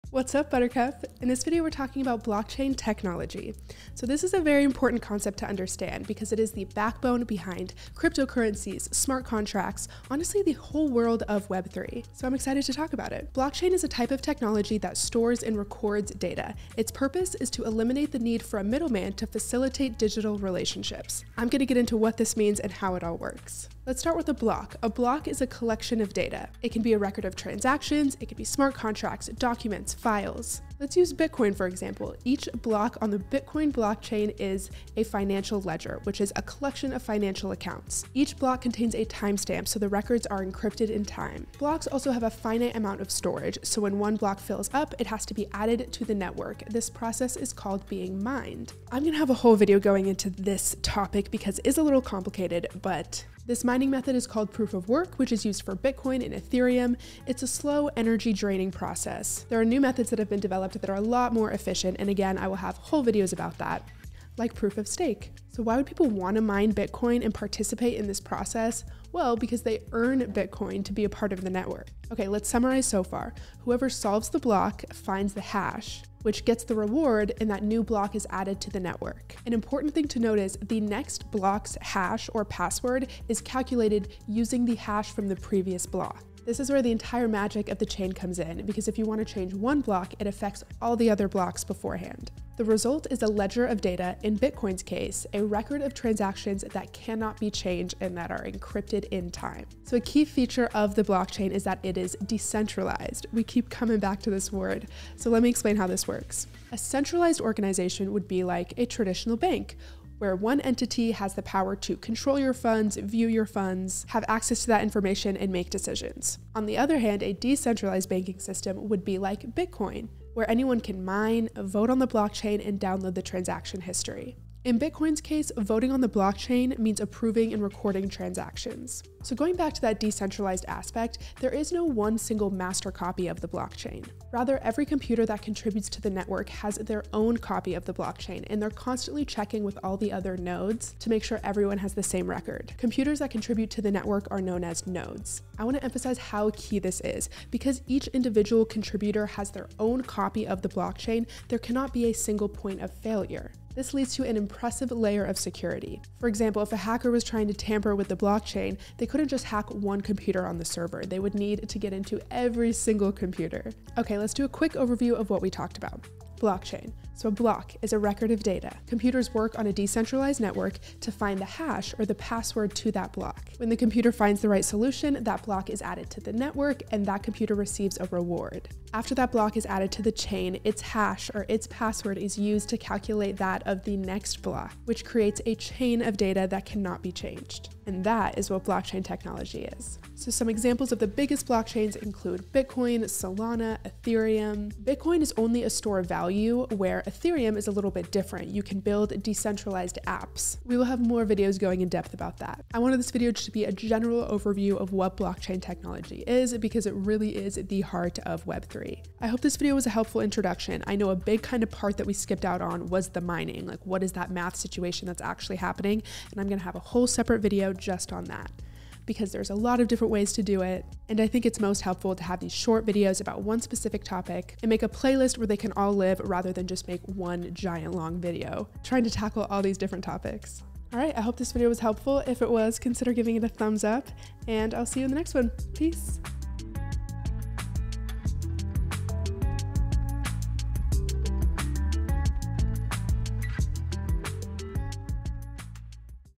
What Is Blockchain Technology?
Blockchain technology is an important concept to understand in the Web 3.0 space, this video serves as a broad explanation and introduction for how the tech works.

If you want to be on the podcast, fill out this form!

Solana Wallet: 7DDeg3sfiDCLWvHj65ZtBXzVMq6p1EfauAZd6KUHnzm9

Ethereum Wallet: 0x4d3945Ce57E29a11412e8CFedA2a4E5dA305C413